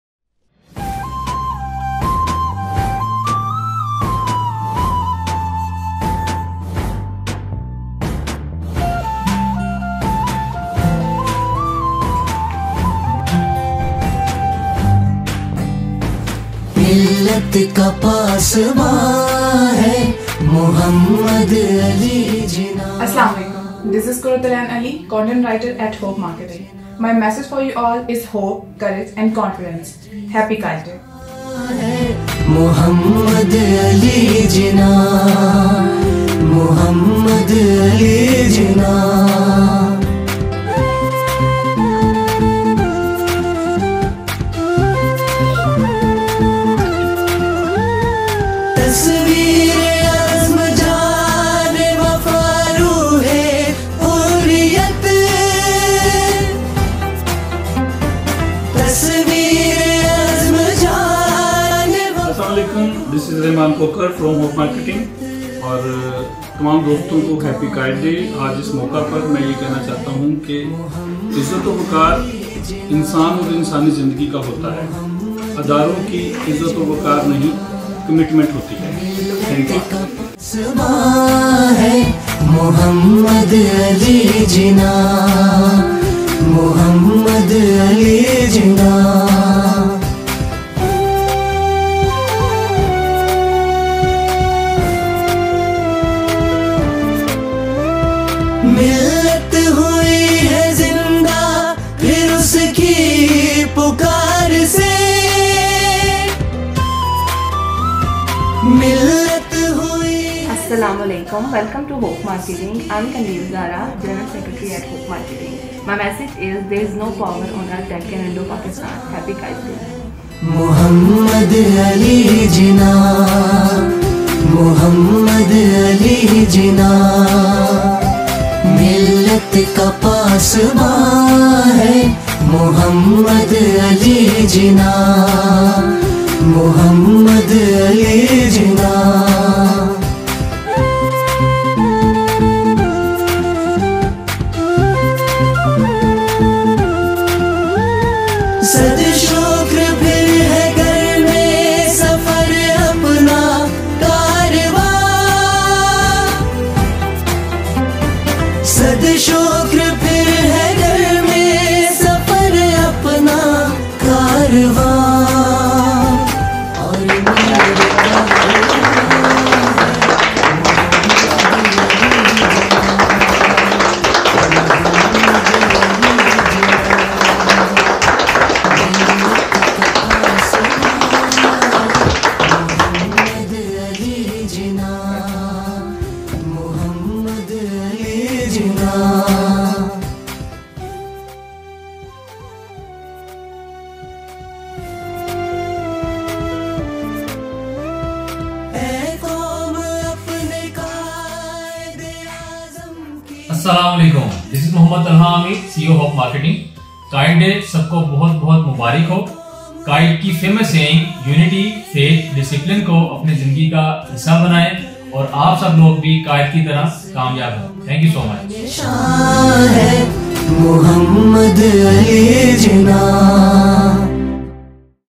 Quaid-e-Azam Muhammad Ali Jinnah Day Celebrated 25 December || HOPE Marketing
HOPE Marketing celebrated the birthday of our national hero and the man of his words Quaid e Azam Muhammad Ali Jinnah
Who told us to live the life according to stay United , by faith in Allah almighty and discipline of Sunnah to persue life in terms of success.
HOPE Marketing pays the tribute to Father of the Nation to give us a free homeland.

Contact Detail:
HOPE Marketing
Office Address: Wallayat Complex, Plaza 74, Bahria Phase 7, Islamabad, Islamabad Capital Territory 46000.